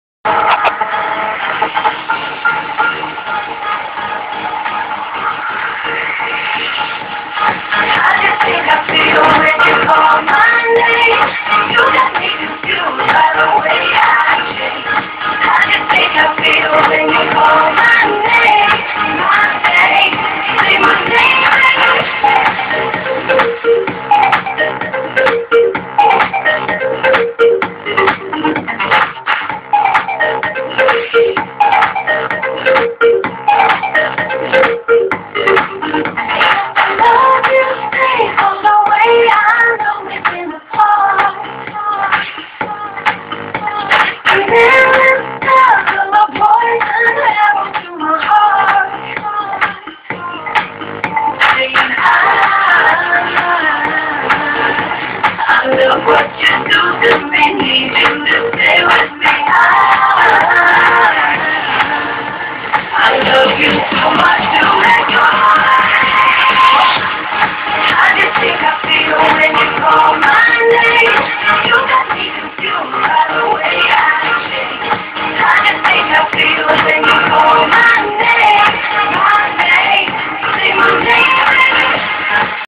HIP HOP SANJAY GADHVI VAGHELA..
ITS MY OWN DANCE VIDEO IN MY VIOLLAGE